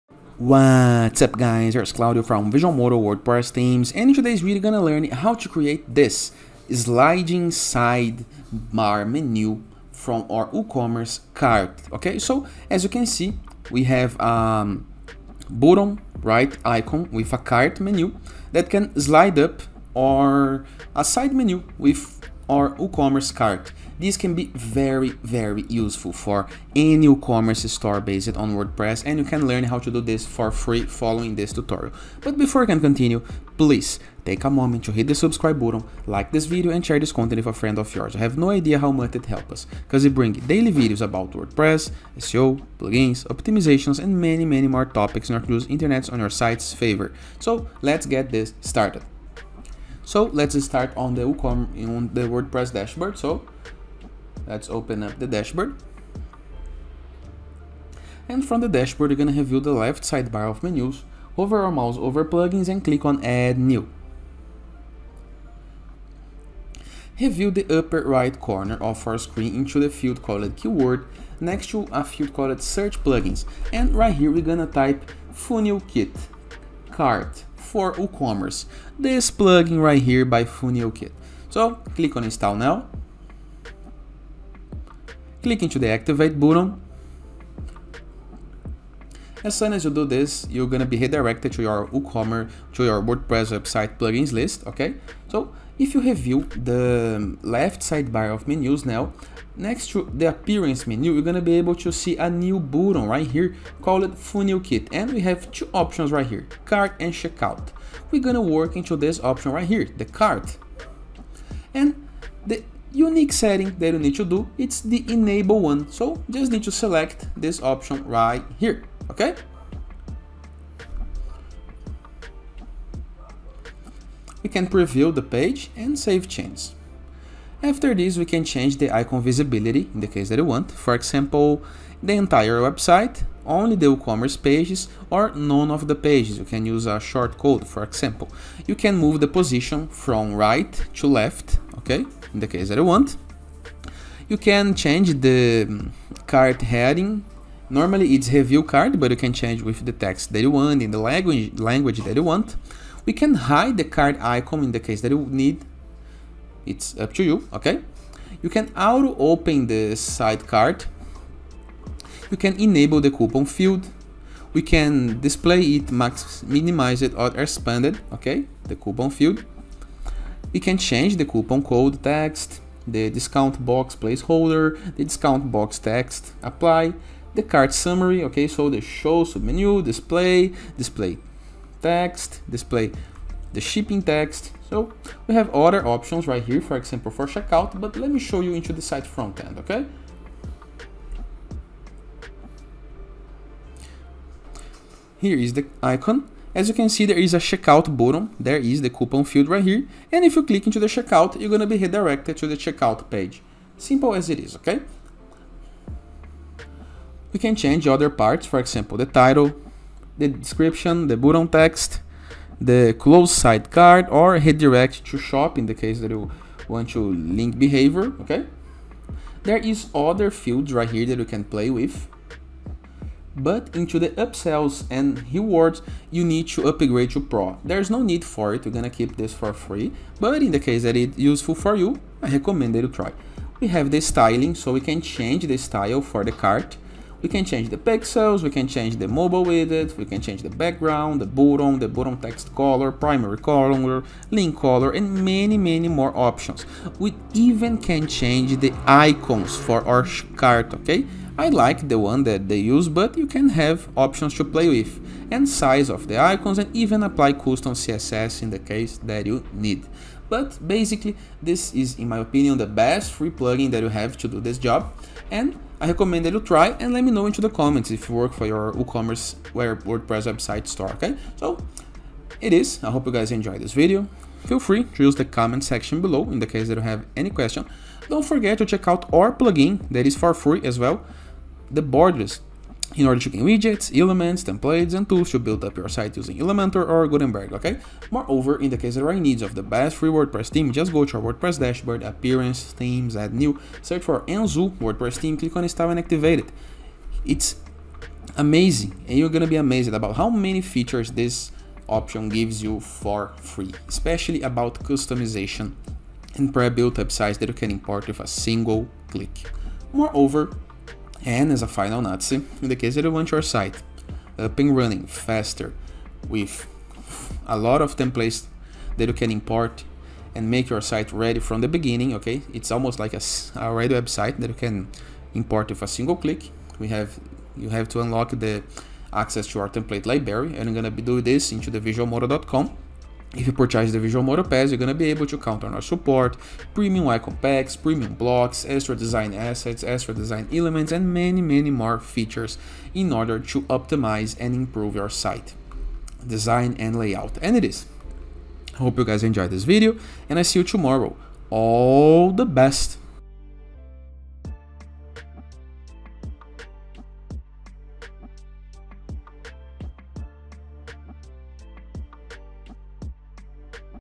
How to Add Sliding Side Cart in WooCommerce WordPress Store - Easily & Free 🛒
In today's video tutorial, we'll learn how to Easily and free add a sliding side cart button and menu on your website WooCommerce shop using WordPress.

Would you like to incorporate a sliding cart into WooCommerce? This facilitates shoppers in viewing their carts from any page on your online store. Customers can then add products to their cart, remove products, and add coupons without having to visit a separate cart page. In this video, we'll demonstrate how to easily add a sliding cart to WooCommerce.

What is the purpose of adding a sliding cart to WooCommerce? A sliding cart on the side is a panel that appears when a customer adds a product to their cart. Customers can also access the cart at any point by clicking on the cart icon. This enables them to verify their shopping cart without having to visit a separate WooCommerce page for the cart.

Depending on the configuration of the cart, shoppers may also be able to add or remove items and directly apply coupons to the sliding panel. You can also include buttons that direct visitors to your WooCommerce's checkout page.

Sliding carts can enhance the customer experience by removing friction from the shopping process, and can help shoppers keep track of their expenditures at the checkout. This can decrease the abandonment rates of carts while also possibly increasing the average value of orders.

The quickest and simplest way to create a cart that slides sideways is to utilize Cart For WooCommerce By FunnelKit. This free plugin facilitates the addition of a cart icon to your website.

Consumers can press the button that activates the sliding side cart. You can alter the color of the cart, its borders, its buttons, its messages, and so on. Many options are available to make it perfectly compatible with your WordPress theme. You can display the cart button across your entire website, limit it to WooCommerce-specific pages, or add the button to specific posts and pages using a shortcode.